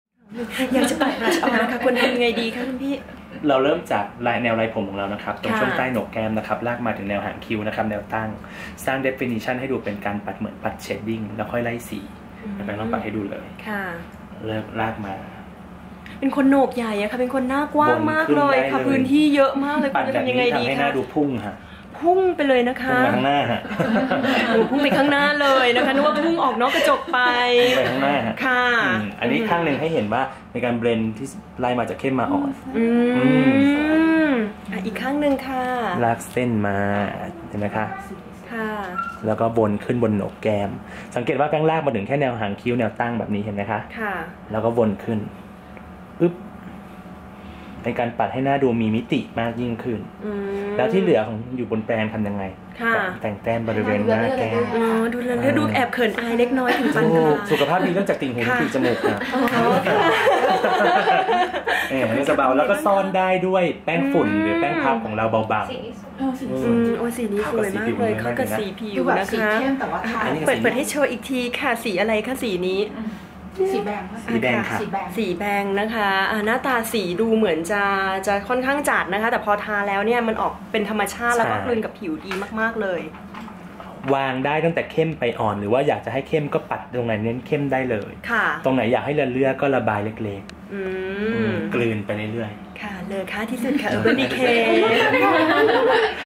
Urban decay แนะวิธีปัดแก้มสำหรับสาวหน้าบาน
ทีมพีอาร์ของ Urban Decay บุกออฟฟิส seventeen เพื่อมาอัพเดตคอลเลคชั่นใหม่ในซีซั่นนี้กันค่ะ ที่เริ่ดสุดเห็นจะเป็นตัวบลัชออนสีเจ็บที่ปัดแล้วหน้าบานๆ ดูเข้ารูปเข้าร่างอย่างไม่น่าเชื่อดดยพี่แฟร้ง เมคอัพเทรนเนอร์ของ Urban decay ค่ะ